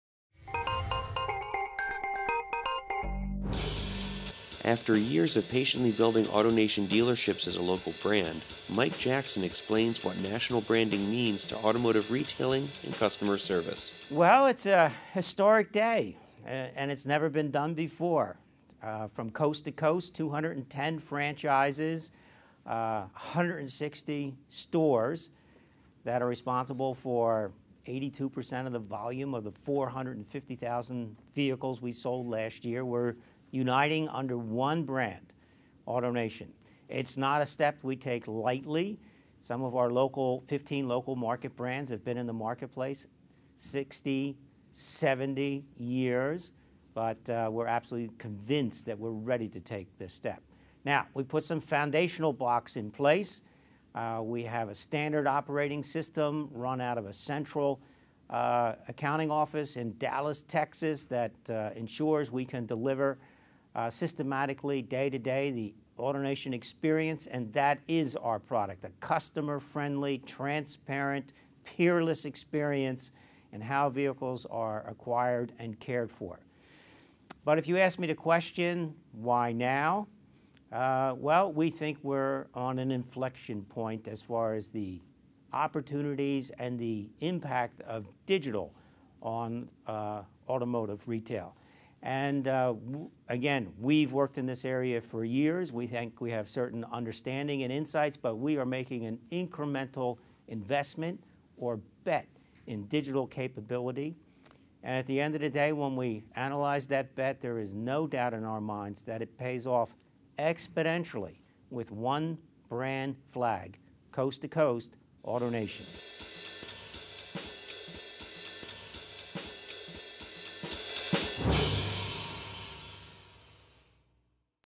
Largest US Auto Dealer Goes Coast to Coast | Mike Jackson | Automotive Digest
After years of patiently building AutoNation dealerships as local brand, Mike Jackson explains what national branding means to automotive retailing and customer service.